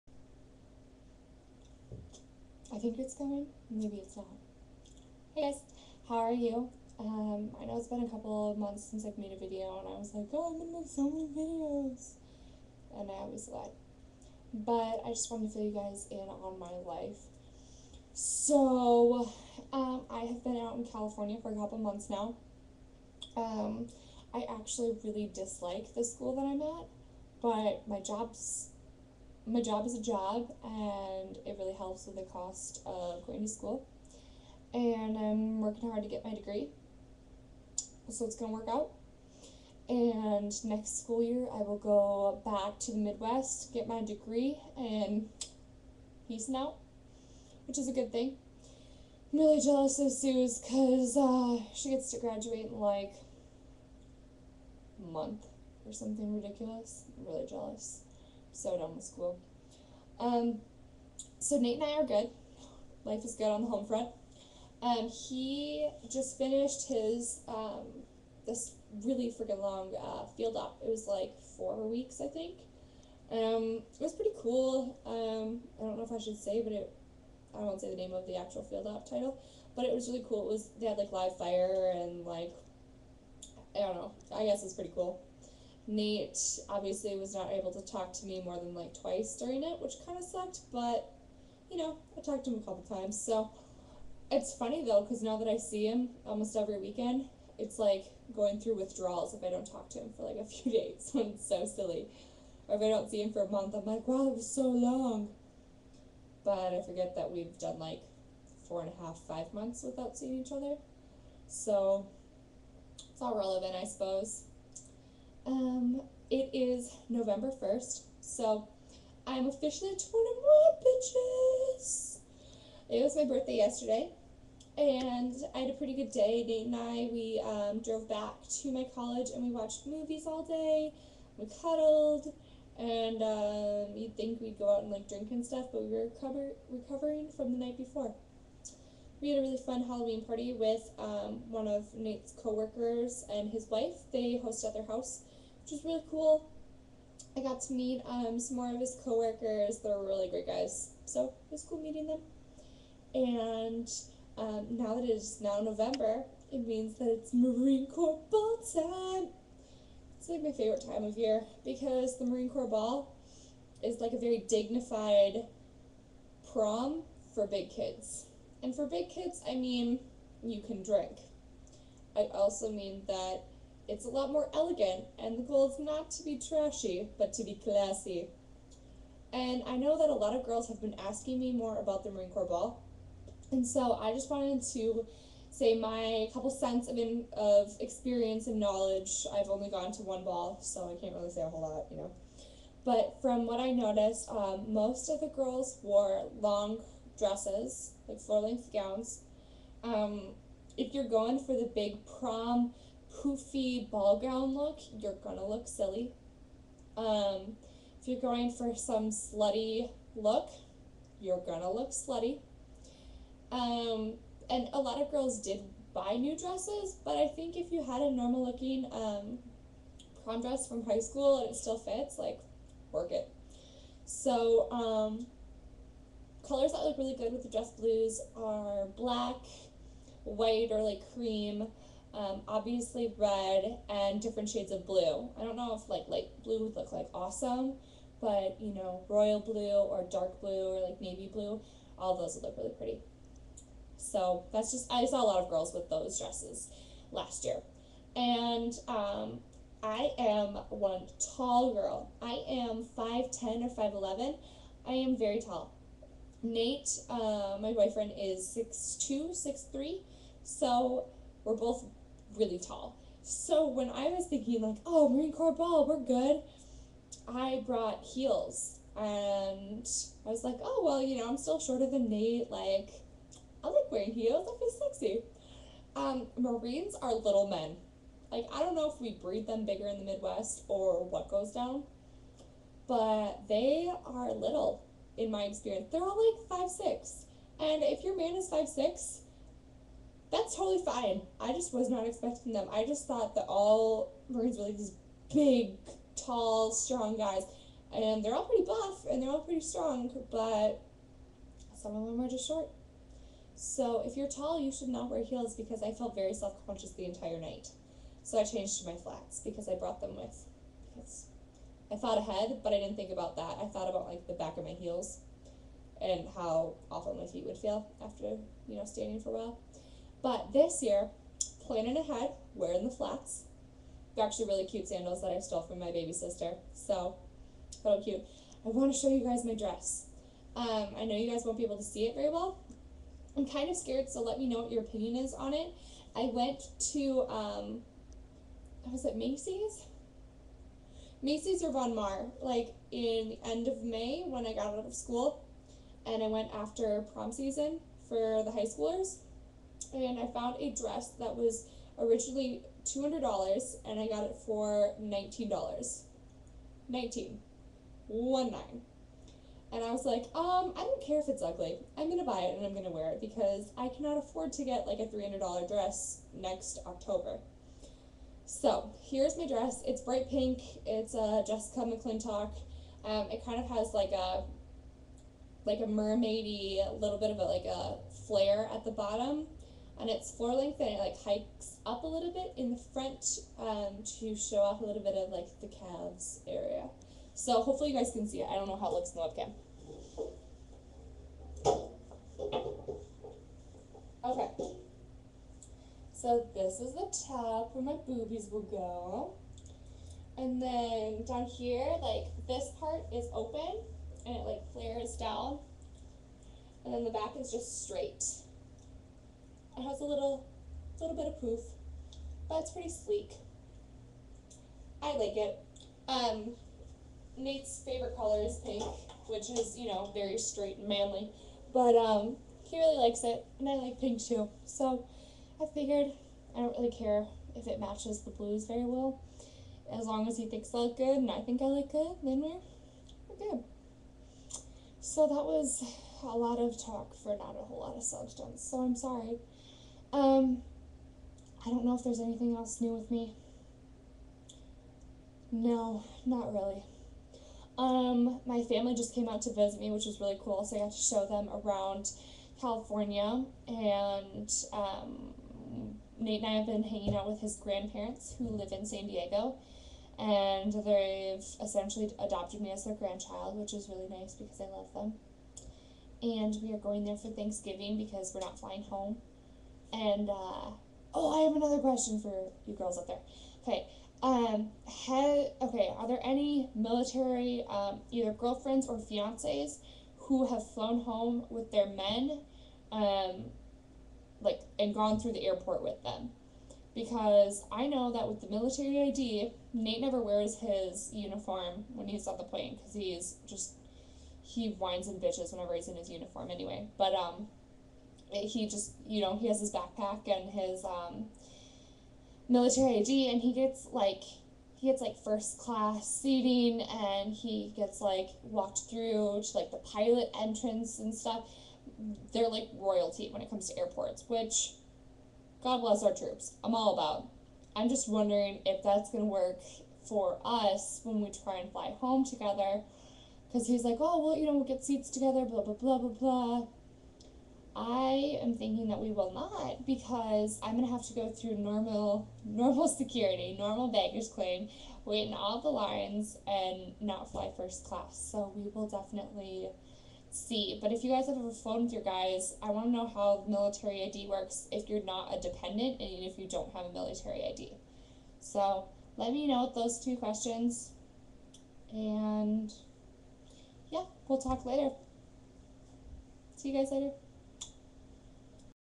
it's time for the marine corps ball again!
bah, I love november. It's time for the marine corps birthday ball and I am SUPER excited. I want to have your guys' opinions on my dress for this year's ball, and also have a question about flying with your military man. Because we are not married, I am not a dependent and am wondering how security and royal treatment our troops receive will impact me as a civilian and our feeble attempt to fly home for xmas together will go. :)